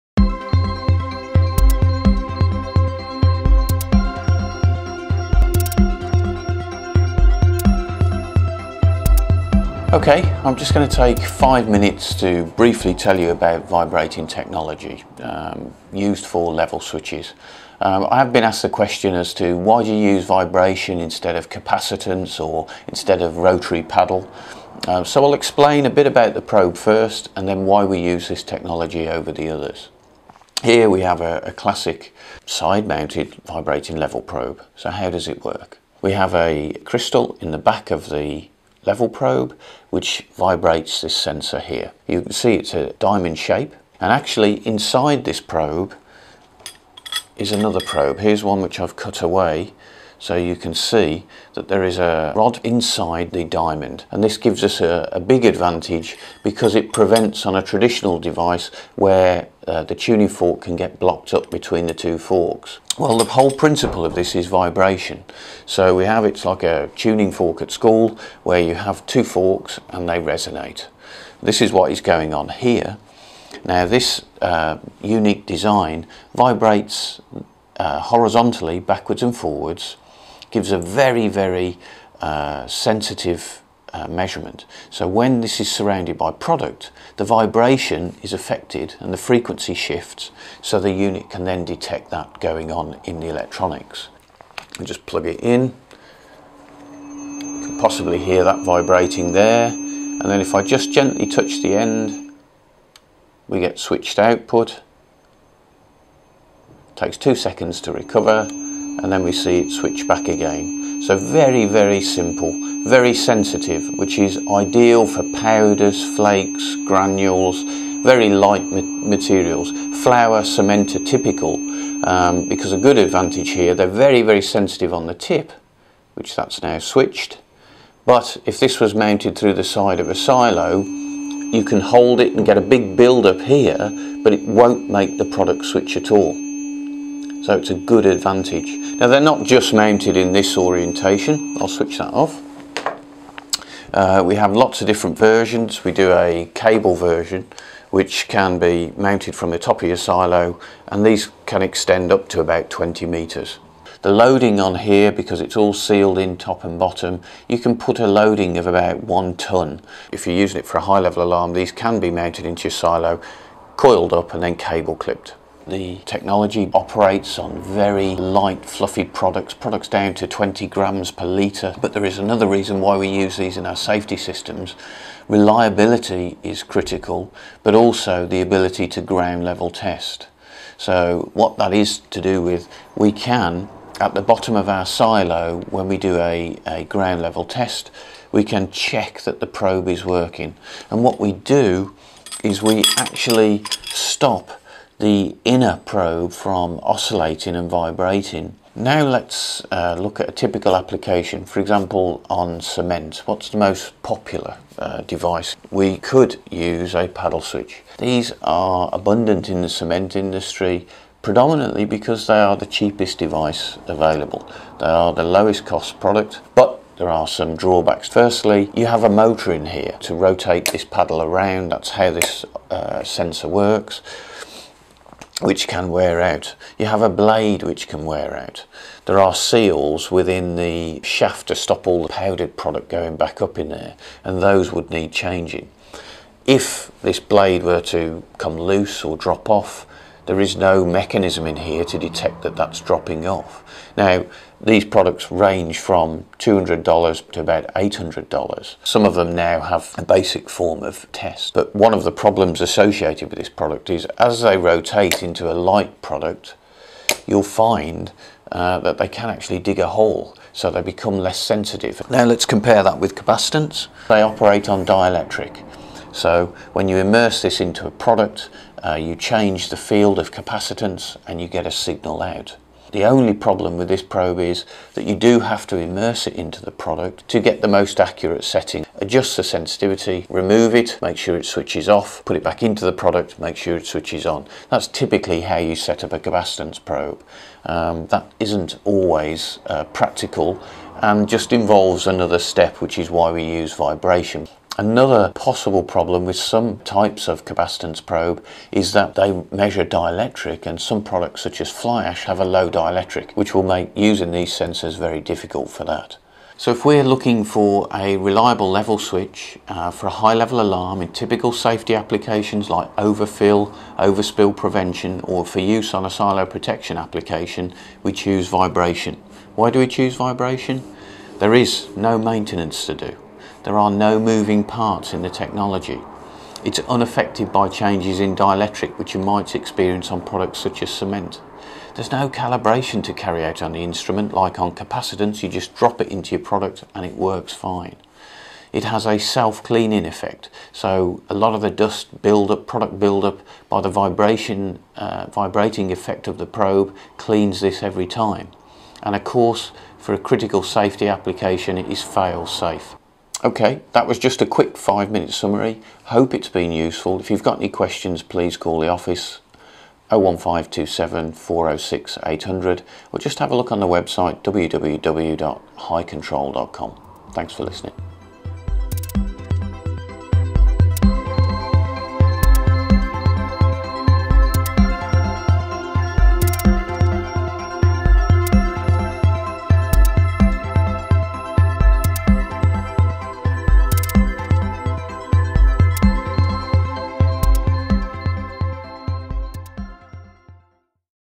Why Choose Vibrating Probes for Safety Systems?
In this video, Managing Director Nigel Allen answers a question we get asked quite often - why do Hycontrol recommend vibrating probes over alternate technologies like capacitance and rotary paddle switches? Nigel explains the unique benefits of vibrating technology in solids and powders applications, compared with the limitations of the alternatives.

This video has subtitles